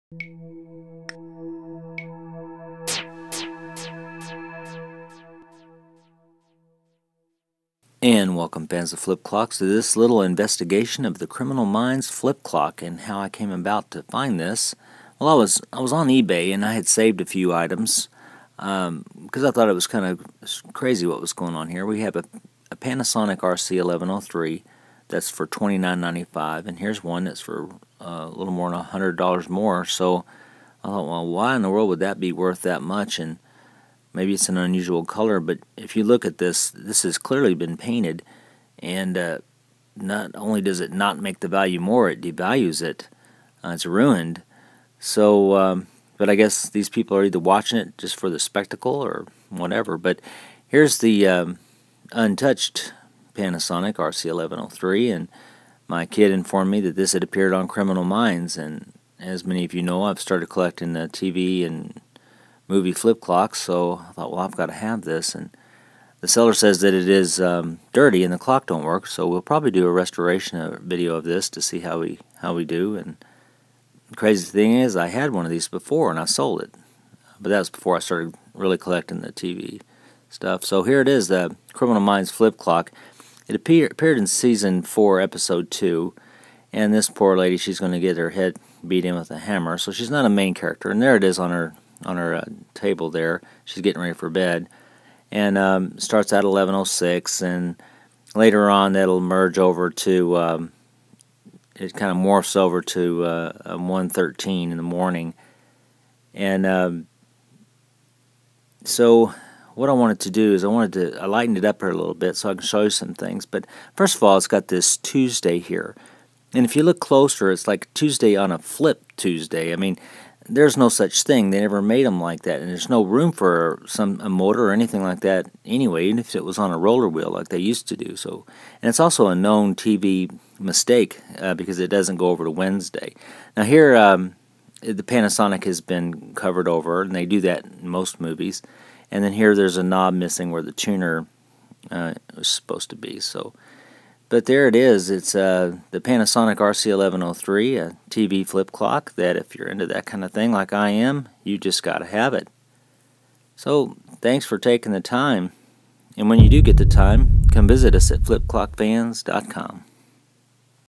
Criminal Minds Flip Clock - Panasonic RC-1103
Introducing the flip clock that appeared on Season 4 Episode 2 of the TV show, Criminal Minds.